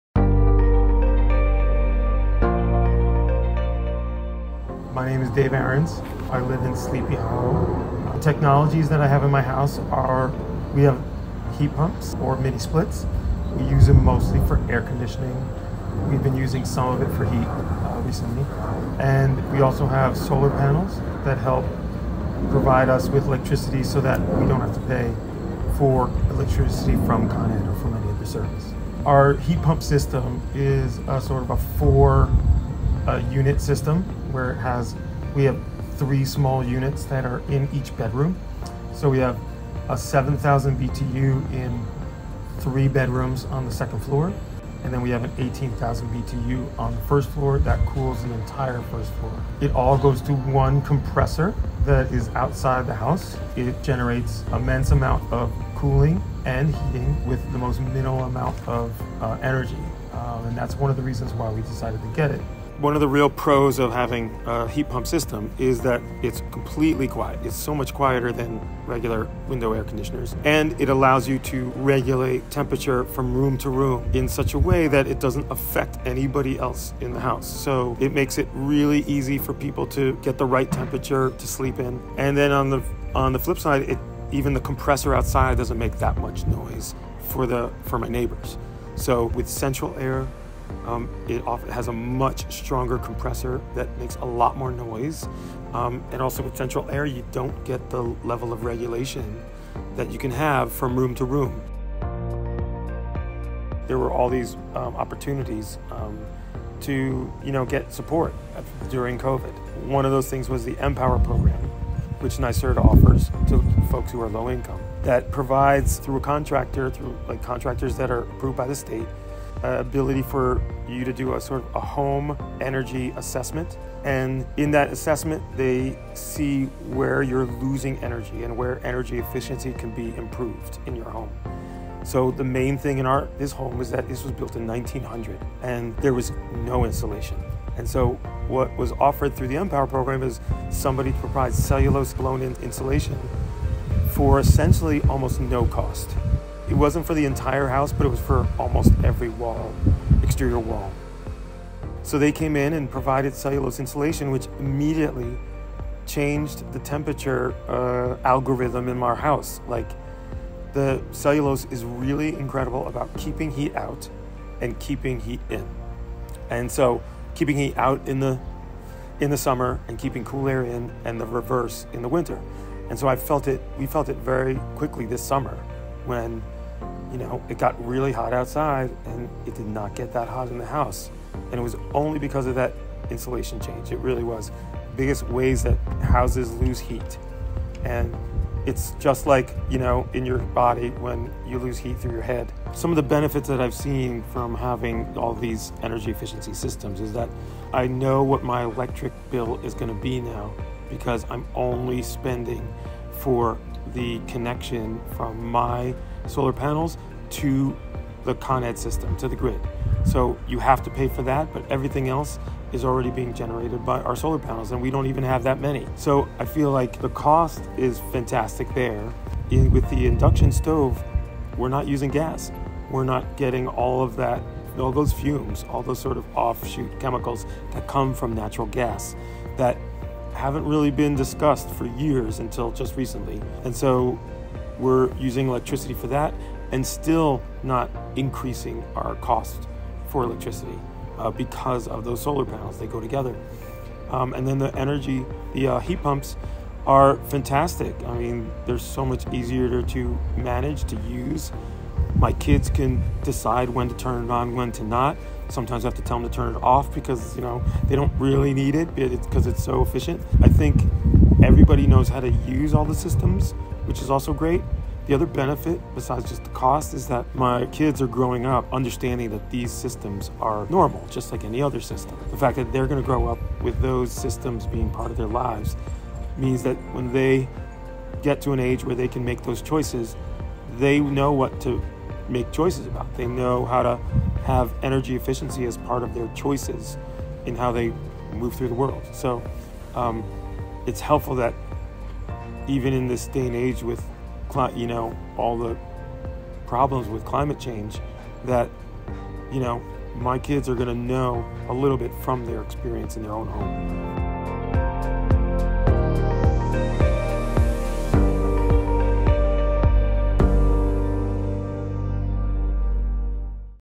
Air Source Heat Pumps + An Induction Stove in Sleepy Hollow
The Arons family moved to Sleepy Hollow in November 2019 and set out to renovate their home’s insulation and air-conditioning systems. Thanks to New York State’s EmPower program, the family was able to pursue these energy-efficient upgrades. The program provides funding for eligible customers looking to make home energy upgrades, and helped the Arons family schedule a free home energy assessment. The family has since installed four air-source heat pumps, and uses solar panels for electricity. These new systems have not only made the Arons’ heating bills more consistent and affordable; they have also helped to build a better, more sustainable future for the family’s children.

To learn more about clean heating and cooling technologies